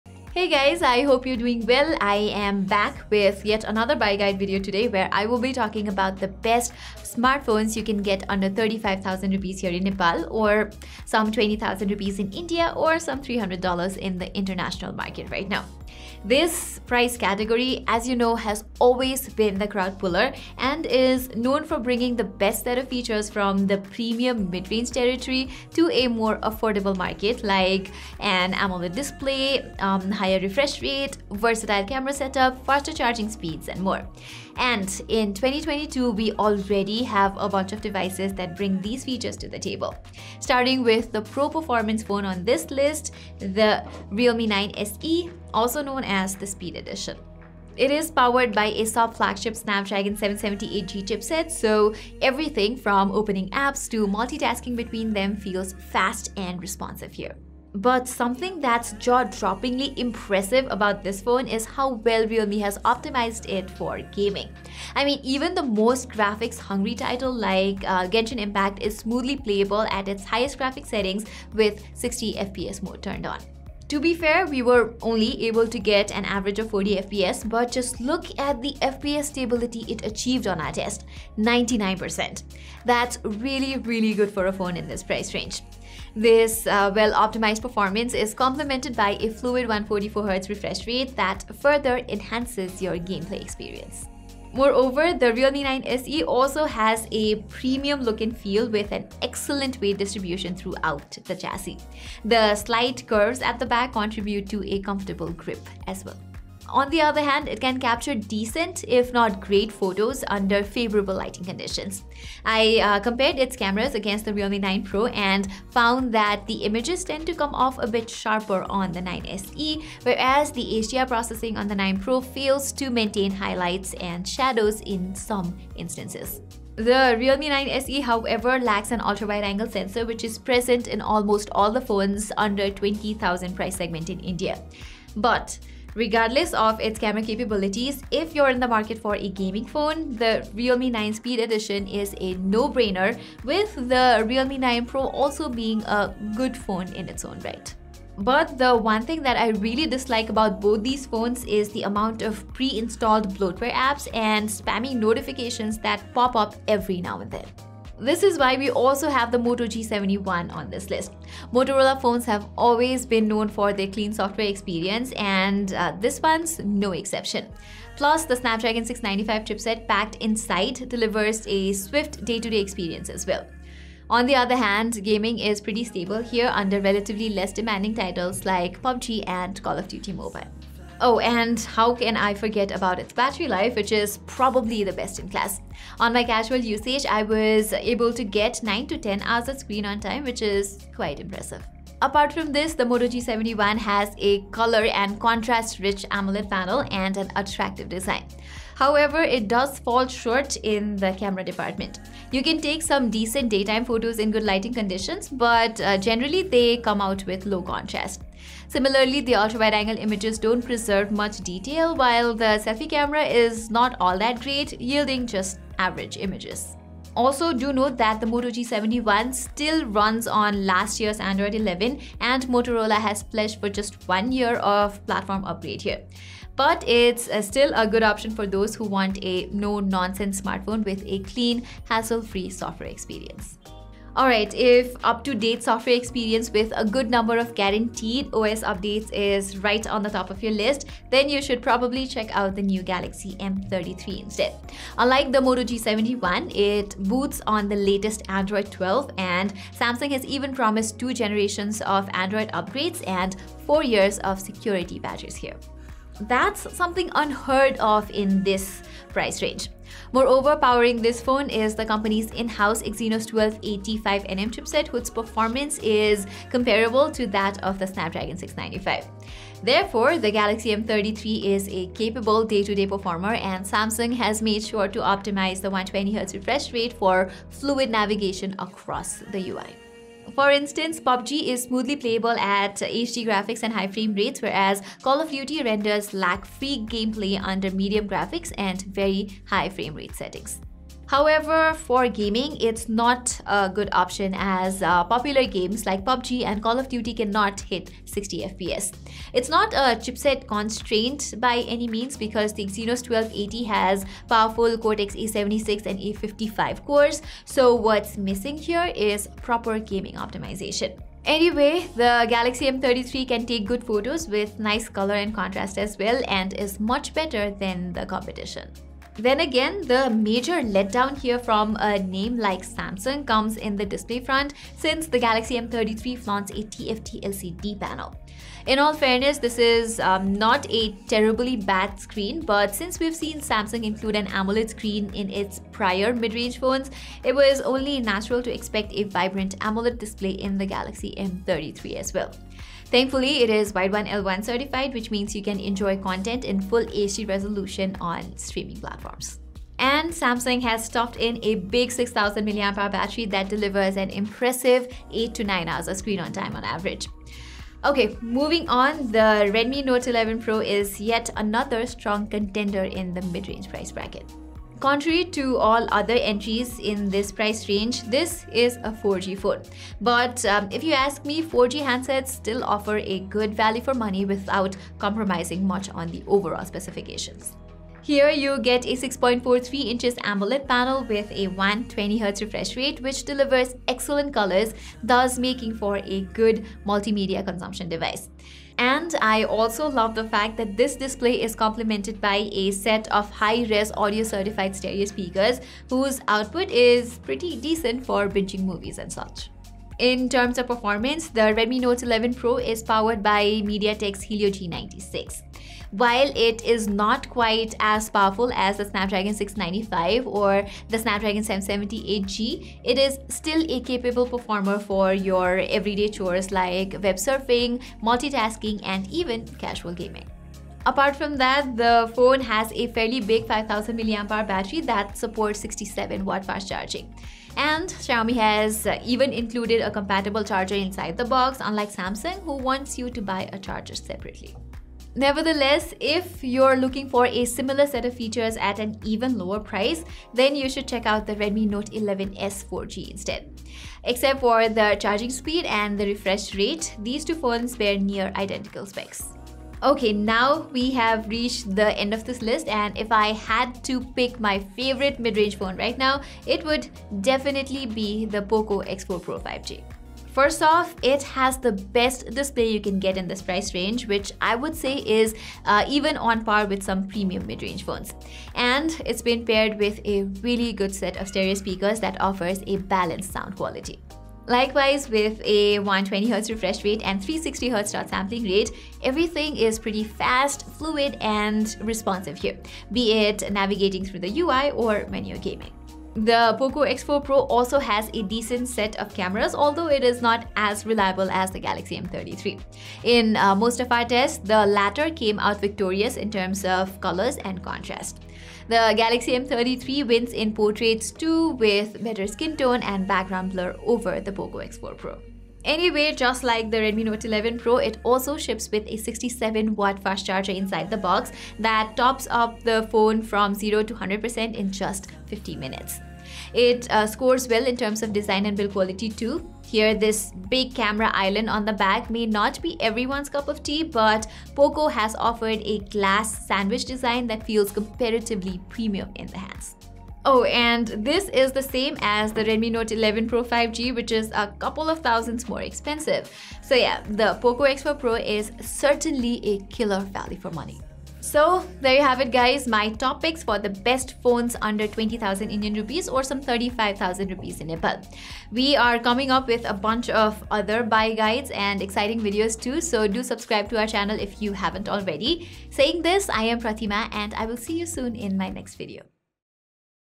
Best Phones to Buy Under INR 20000 | NPR 35000 | USD 300
Hey everyone, in this video we have listed our top picks of phones you can buy under INR 20,000, NPR 35,000, or some $300. Enjoy!

1. Realme 9 SE:
Nepal: Not Available yet

Gears we use:
🔹Secondary lens
🔹Microphone for Audio
🔹Lav Mic
🔹Tripod Head

Chapters:
00:00 - Intro
00:45 - Realme 9 SE 5G
02:37 - Motorola G71
03:54 - Samsung Galaxy M33 5G
05:57 - Redmi Note 11 pro 4G
07:15 - Redmi Note 11s
07:29 - Poco X4 Pro 5G
09:01 - Conclusion